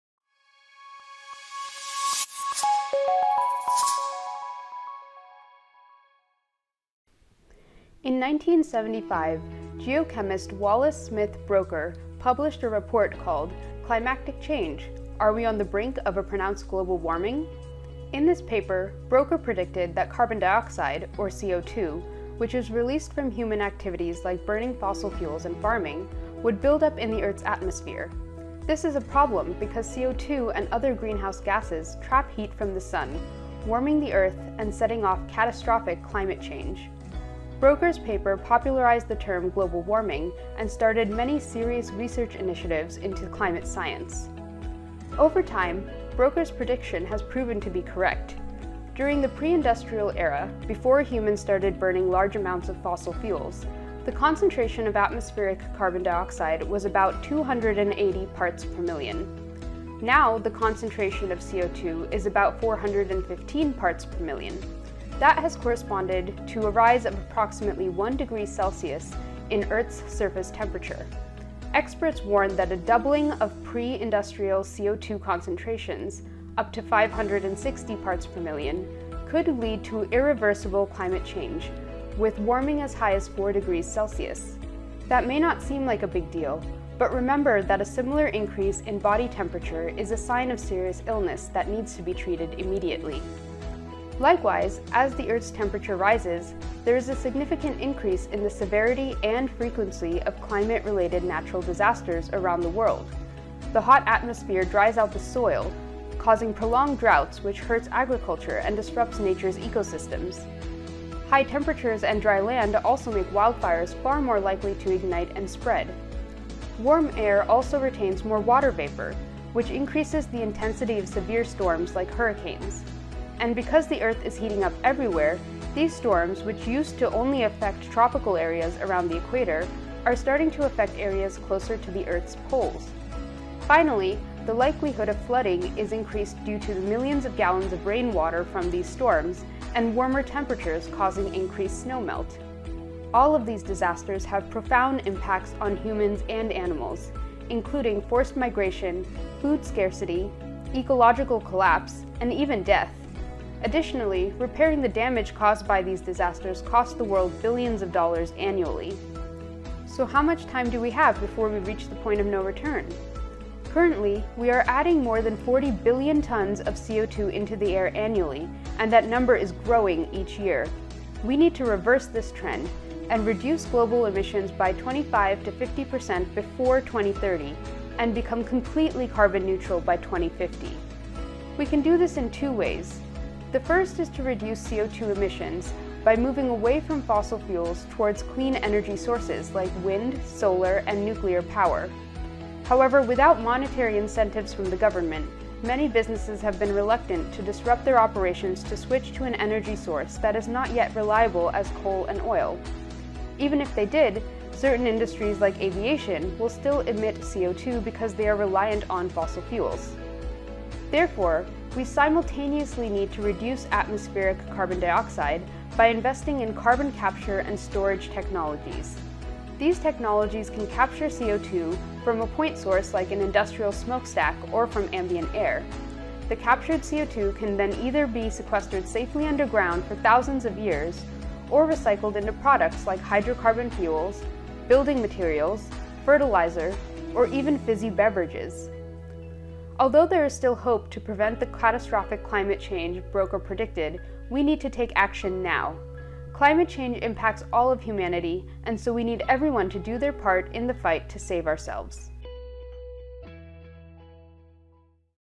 Climate Change: Hype or Hope?
Is climate change really that big of a deal? And if it is, is there anything we can do about it?